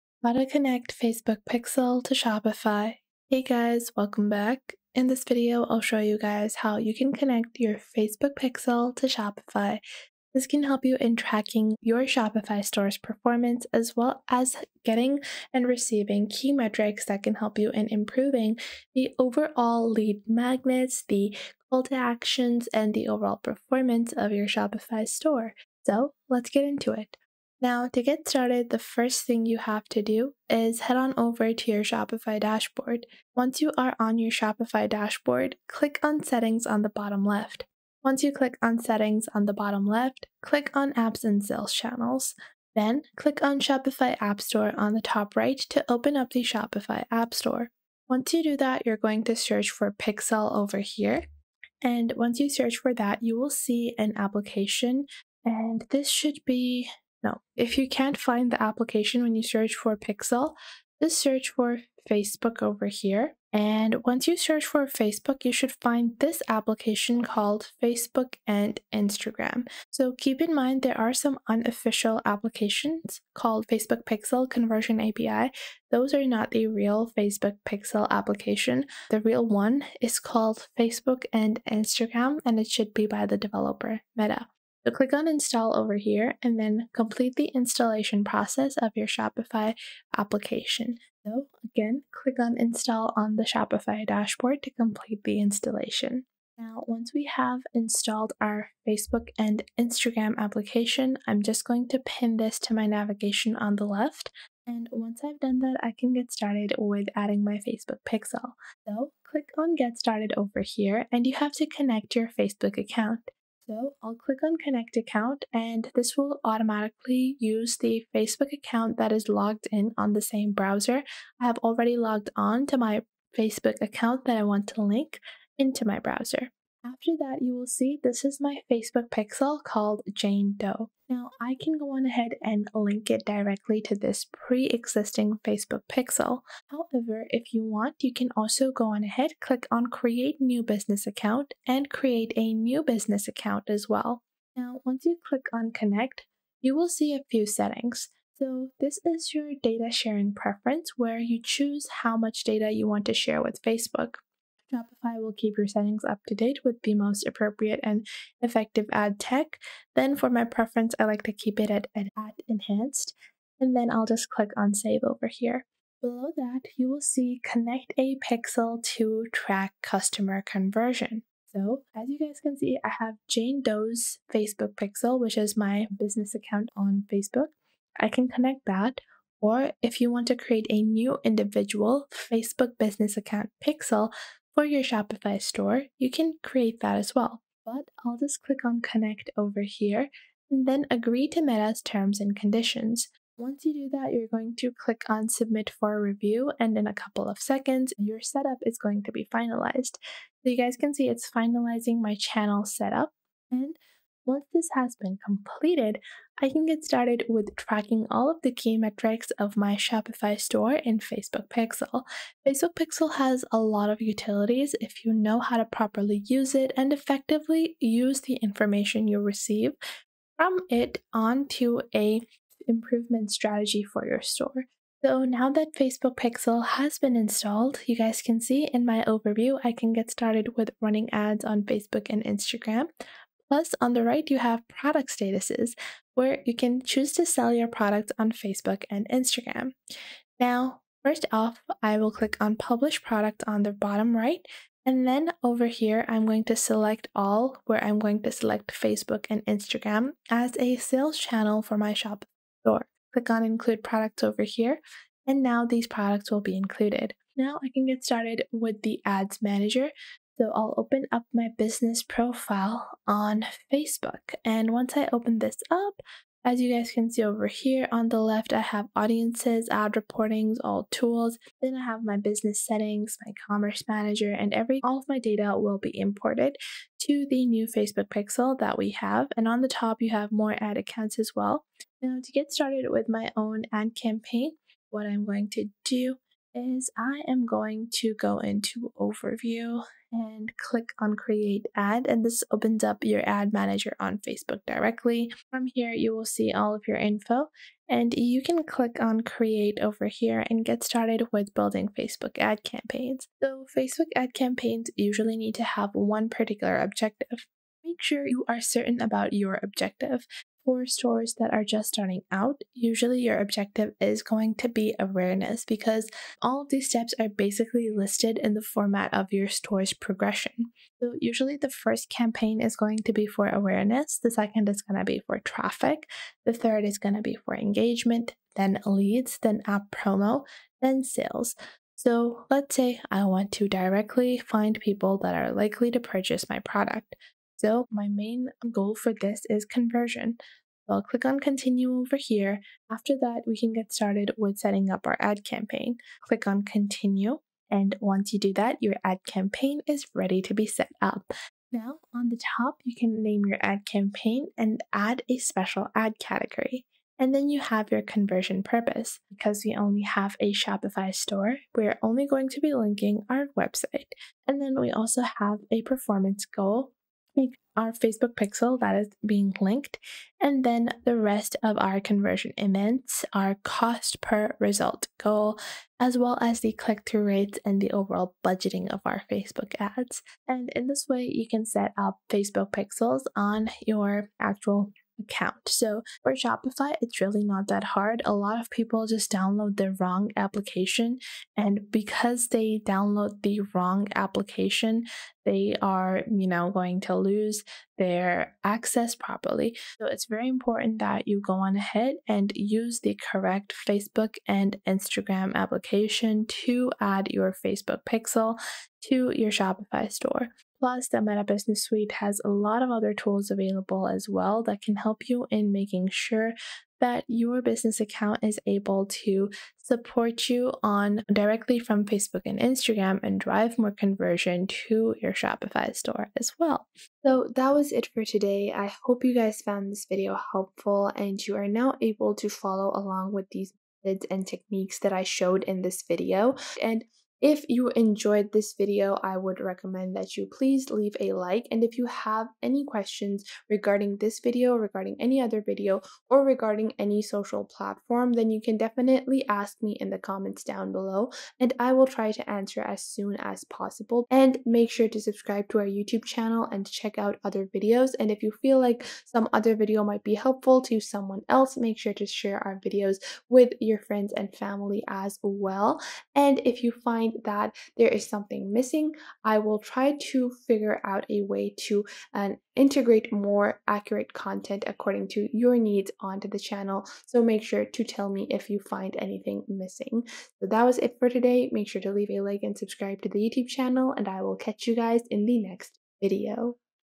How to Connect Facebook Pixel to Shopify (2024)
How To Connect Facebook Pixel To Shopify

In this video I will show you How To Connect Facebook Pixel To Shopify. This video will cover everything that you need to know and help you get started with this software.

Kind regards,